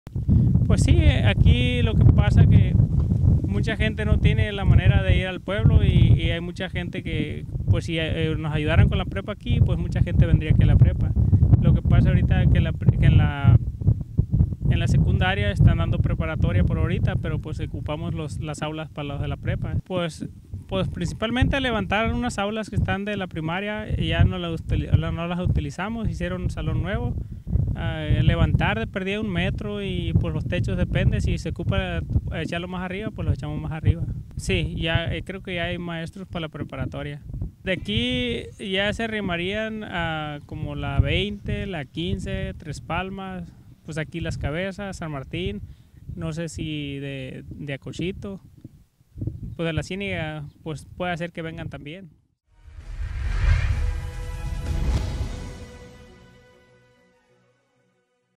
Buscan apoyo para instalar Preparatoria en Las Cabezas
Guamuchil, Sin.-  El ciudadano explicó que en breve estarán buscando el apoyo en las instancias correspondientes para que este proyecto aterrice y así brindar apoyo a cientos de estudiantes de las comunidades de Salvador Alvarado.